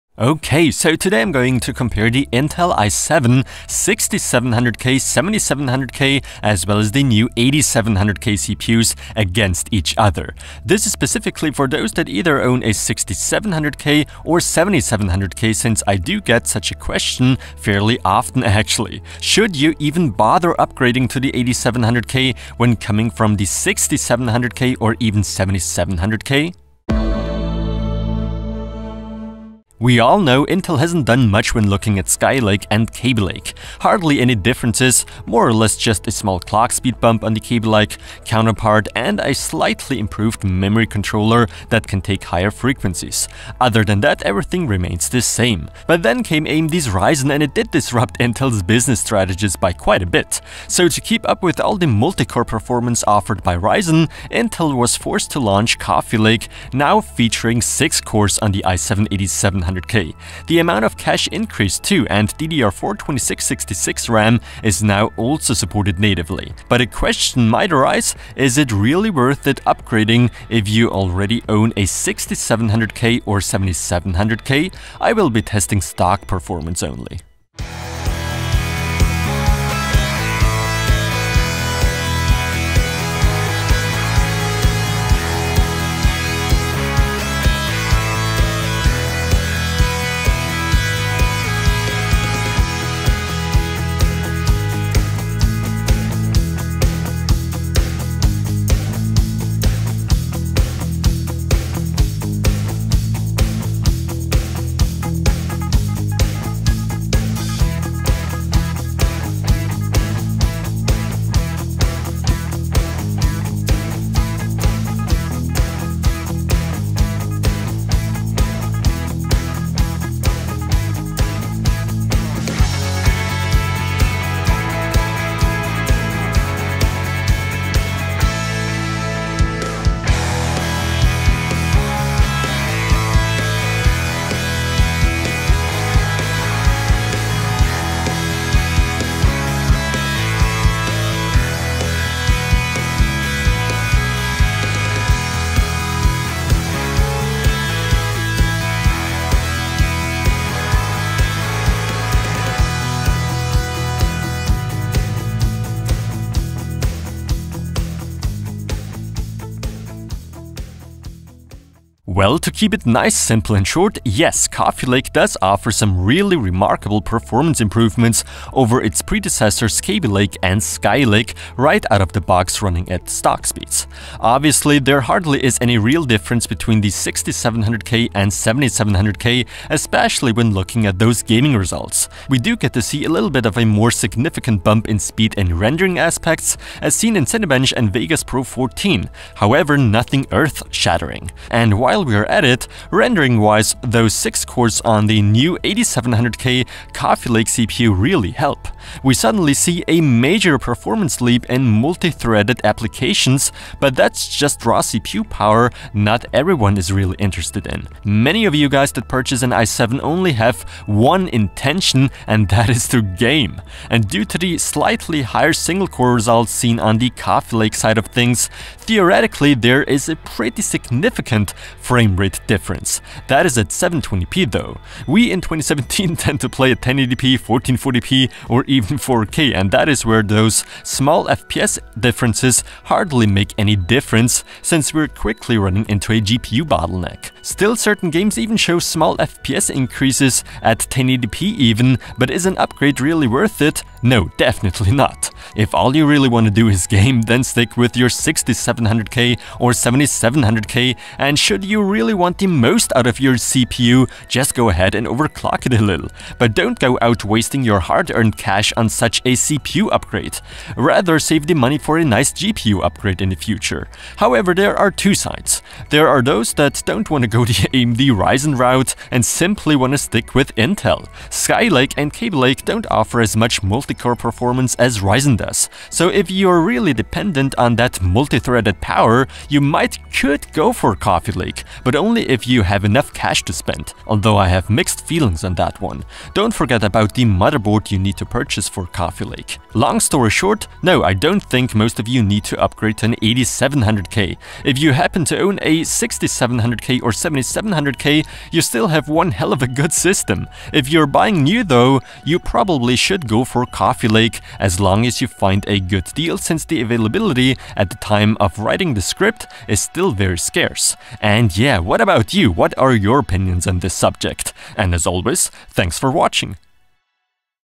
Intel i7-8700K vs 7700K vs 6700K - Should You Even Upgrade?
Intel i7-8700K
Intel i7-7700K
Intel i7-6700K

Intel i7-8700K
Intel i7-7700K
Intel i7-6700K

Is it even worth it upgrading from an i7-6700K or 7700K to the new 8700K?